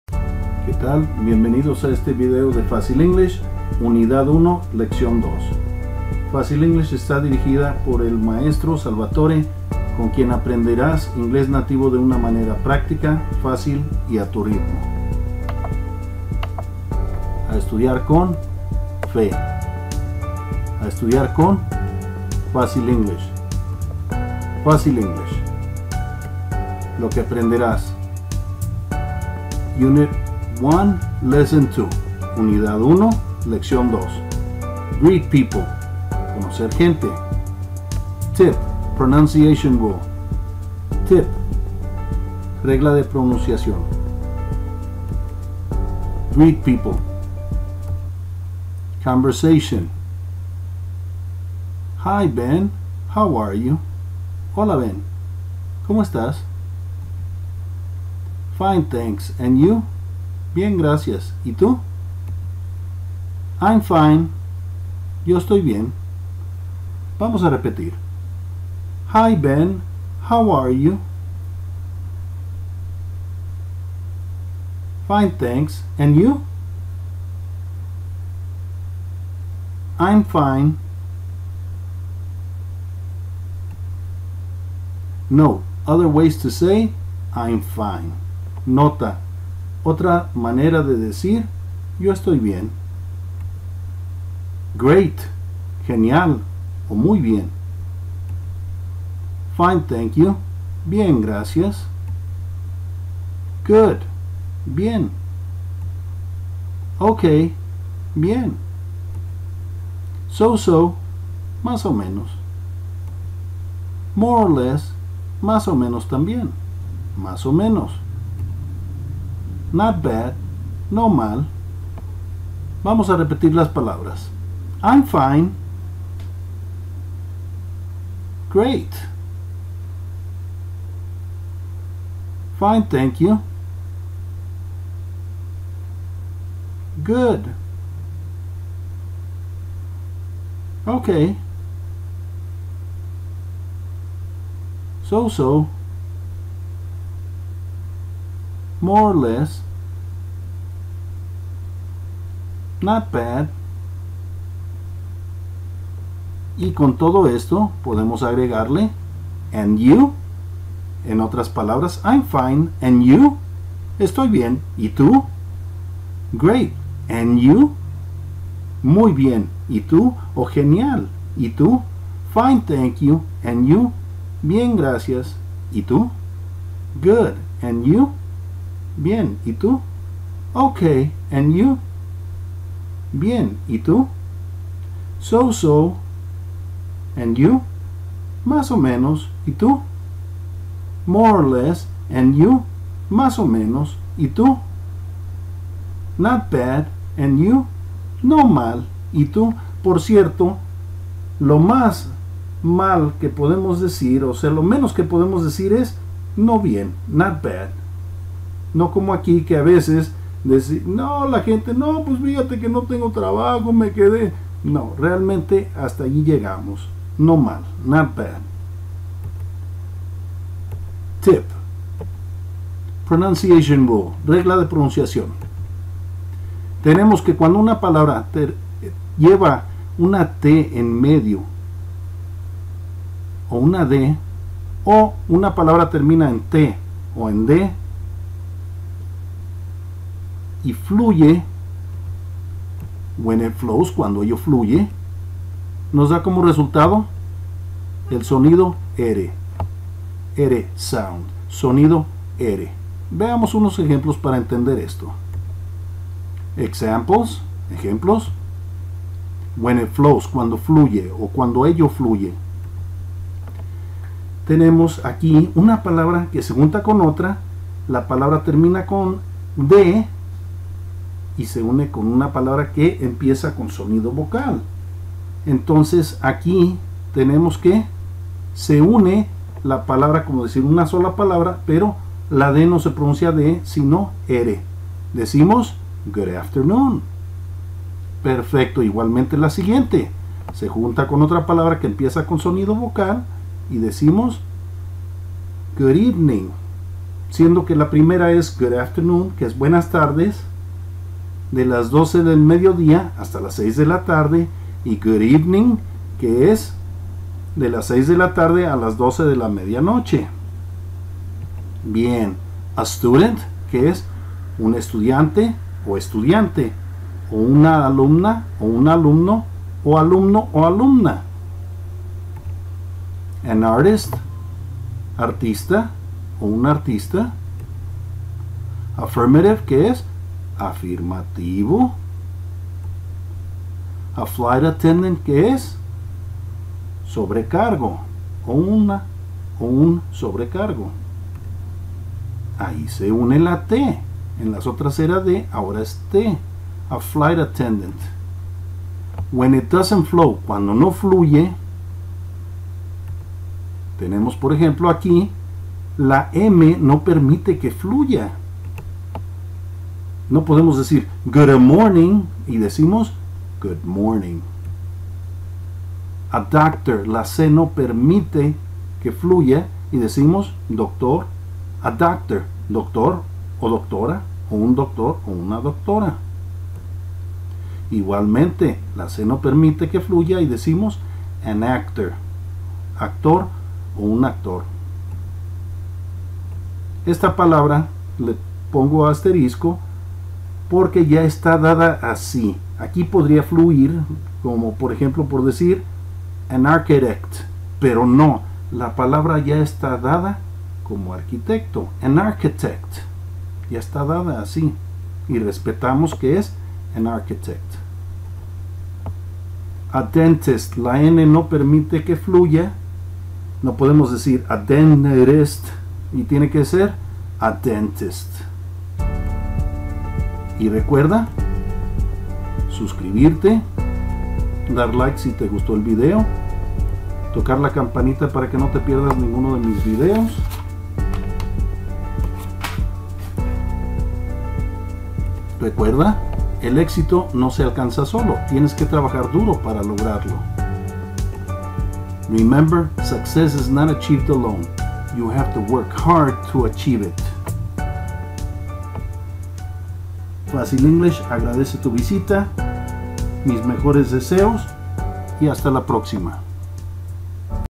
Aprende Inglés Rápido y Fácil. Curso De Inglés Completo.
* Aprende Inglés Nativo Americano desde cero con Fácil English.
Son clases cortas y sencillas. Clases dirigidas por el maestro
Sañvatore.